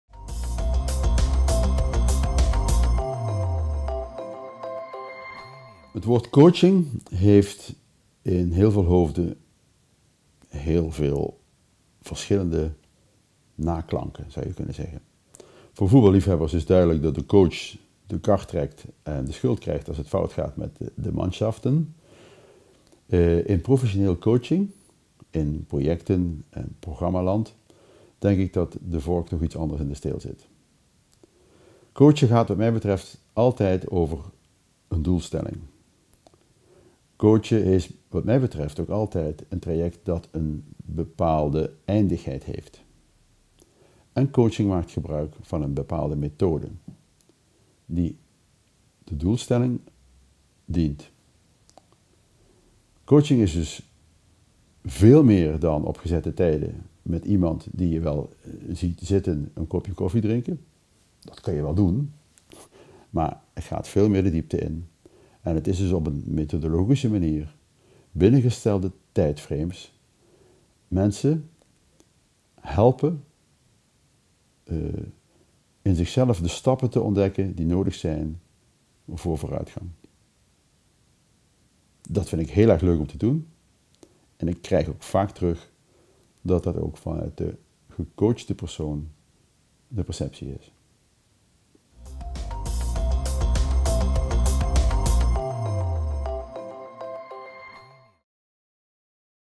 Han Stouten on Coaching
'Coaching unplugged'
Korte 'vlog' over het coachen van professionals door  Han Stouten
Short 'vlog' on coaching professionals by Han Stouten